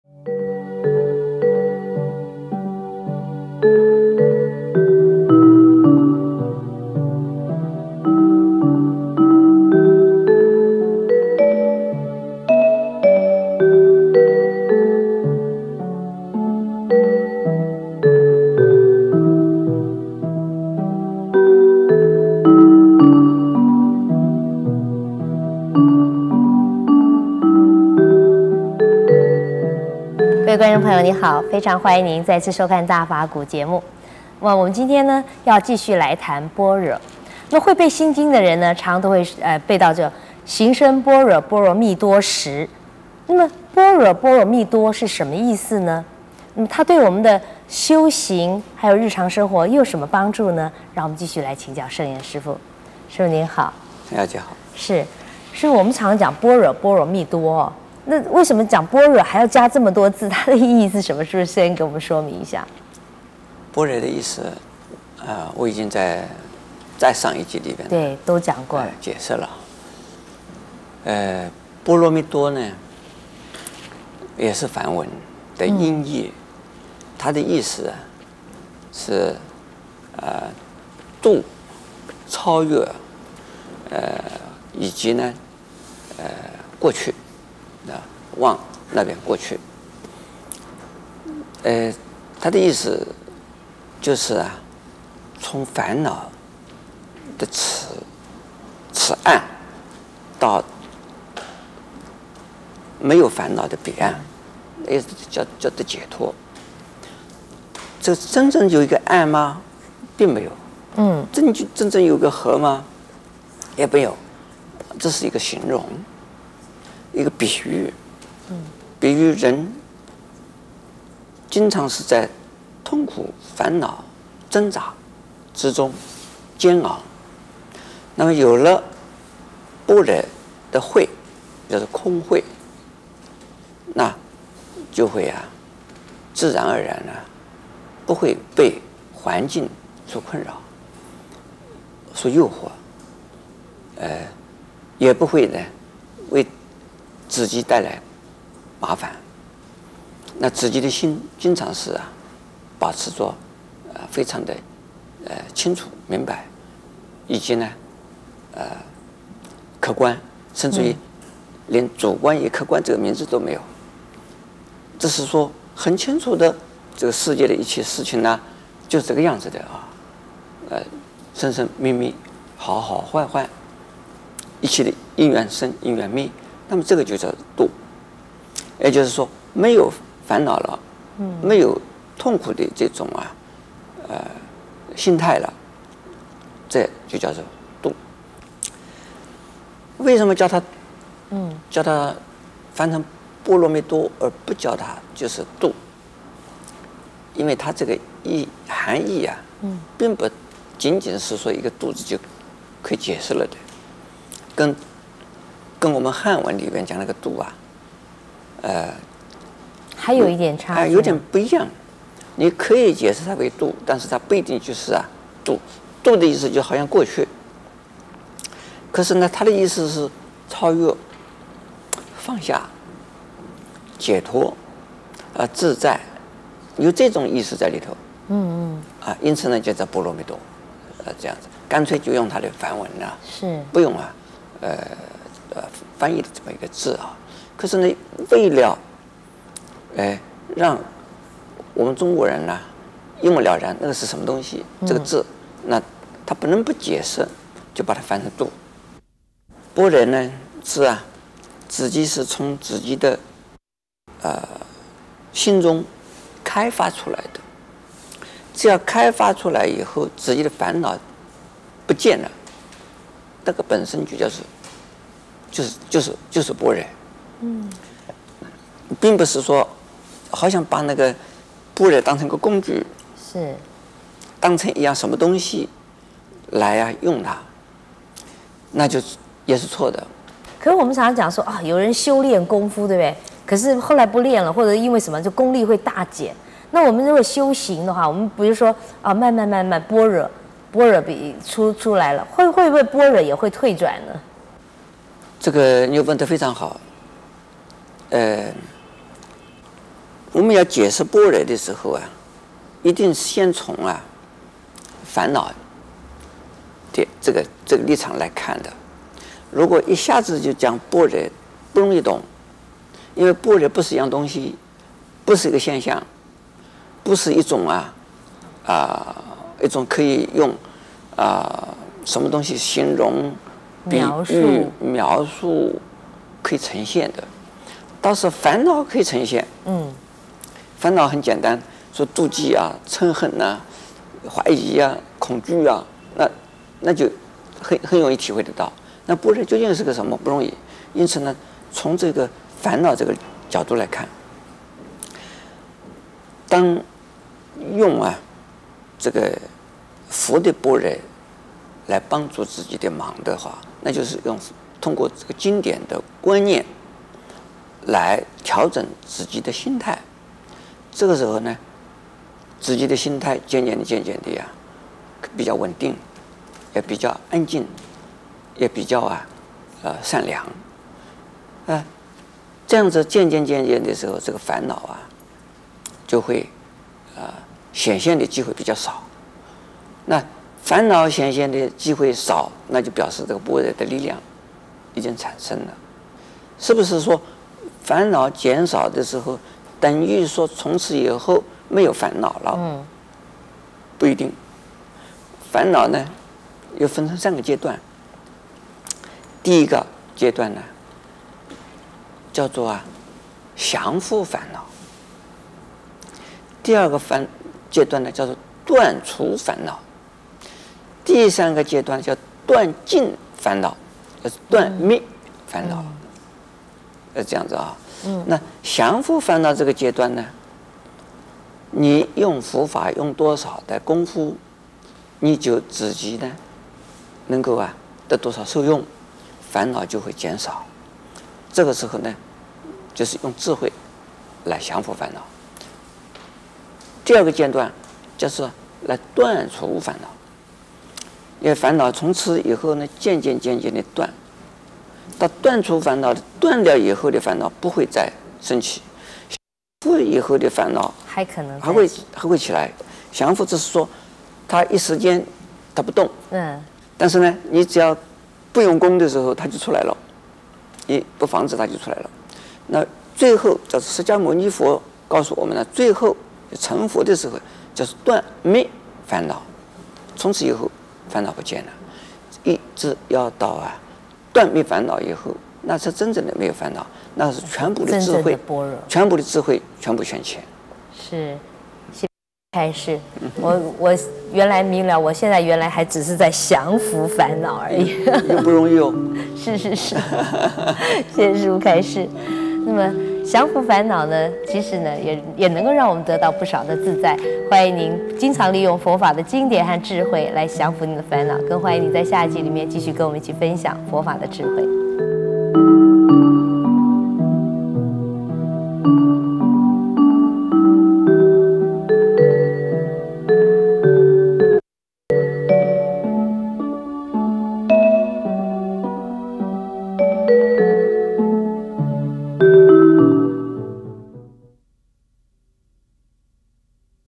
The true meaning of prajna-paramita(GDD-1106)DVD
By developing our prajna, transcending and letting go of our afflictions, we can eventually subdue, reduce our afflictions, and finally eliminate them.                                                                                More online courses